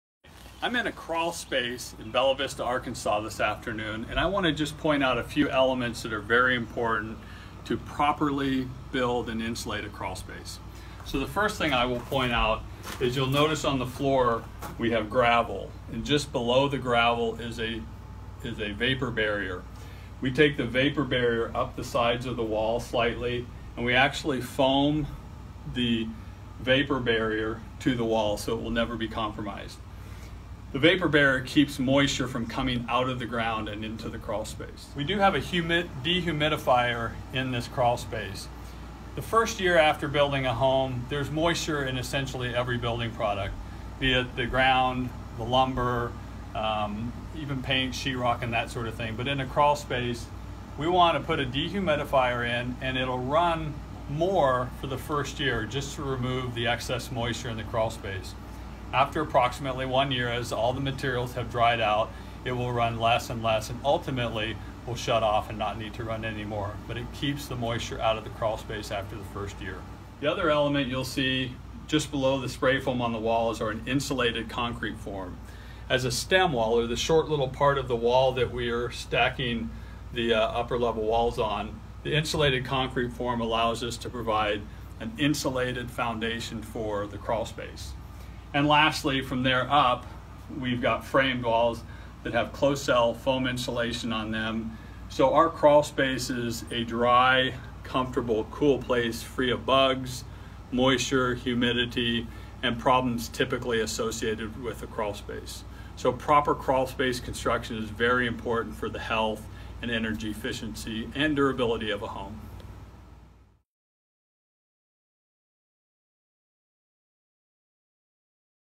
Proper Crawl Space Construction
When designing a new home, most people don't give much thought to the crawl space. But when it starts to retain moisture, produce an unpleasant odor and affect your health, it becomes a top priority. In this video, David Stitt explains how we build and insulate the crawl space for optimal health, energy efficiency and durability of your home.

TheStittGroup.com